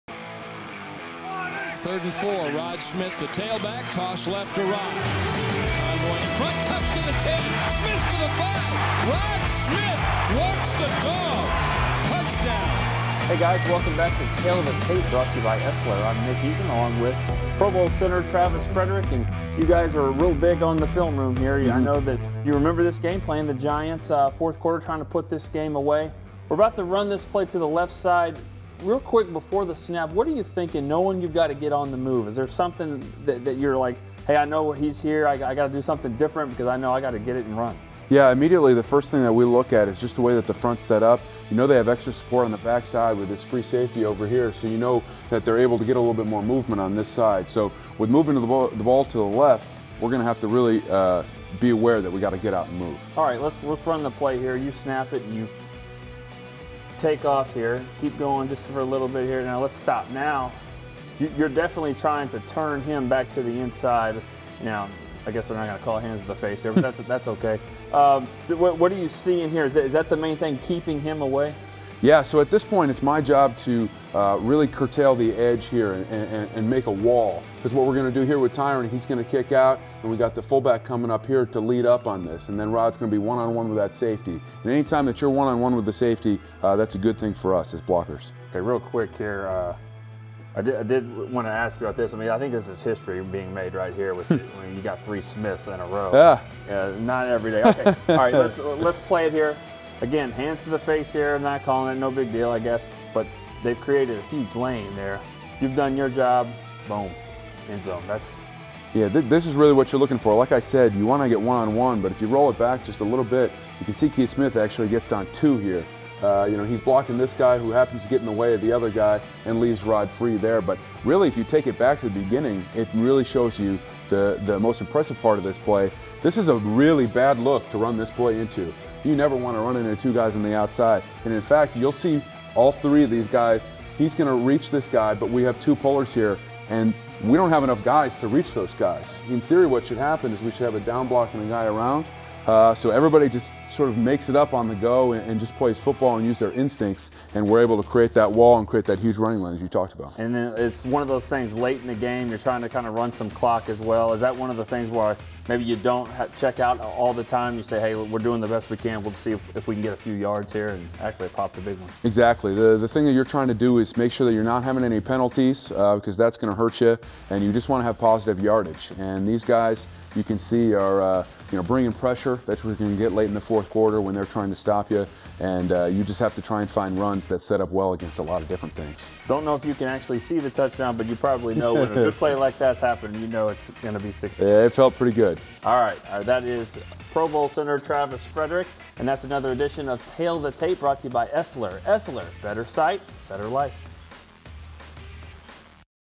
Tale of the Tape: Travis Frederick | Dallas Cowboys 2018
Dallas Cowboys Pro Bowl center Travis Frederick joins Nick Eatman to break down a play from 2017 against the Giants.